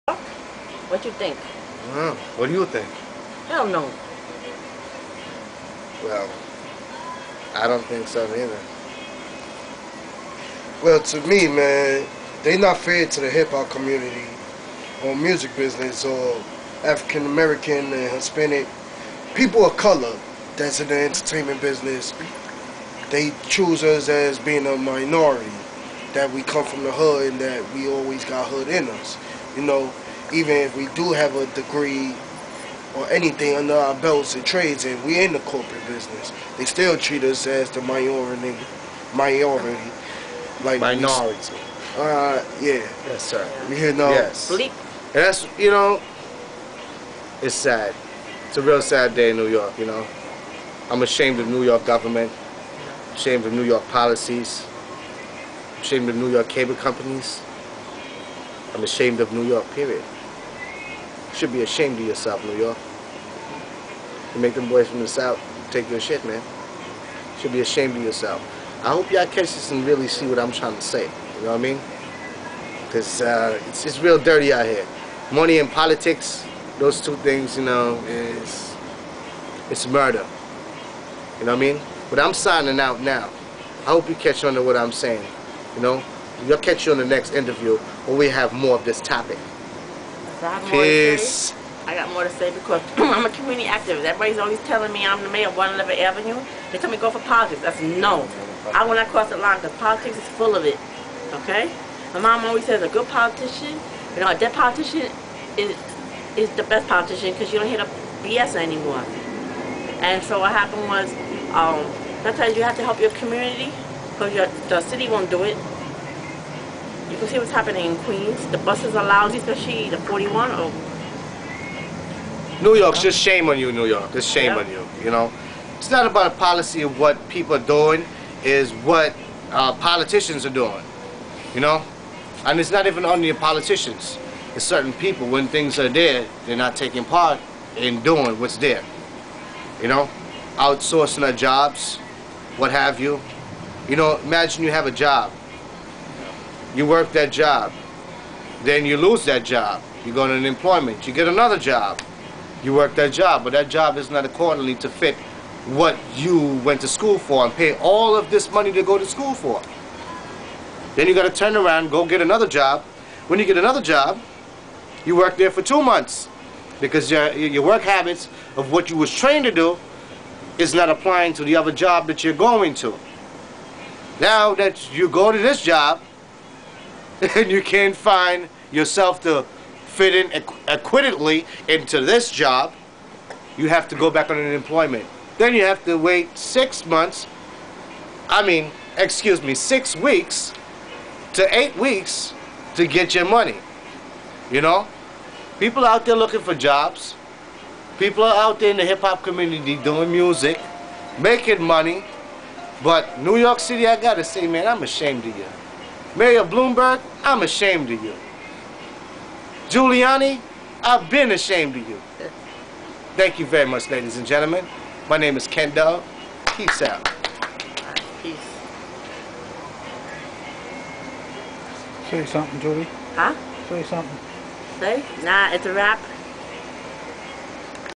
MOV02750 DOES NEW YORK POLITICS HELP HIP HOP COMMUNITIES?
I DECIDED TO AIR THIS ESPECIALLY SINCE POLITICS ONLY DEALS WITH AREAS OF THEIR OWN INTEREST, NOT NEIGHBORHOODS WHICH LOSE THEIR FUNDING BECAUSE OF BUDGET CUTS, ETC.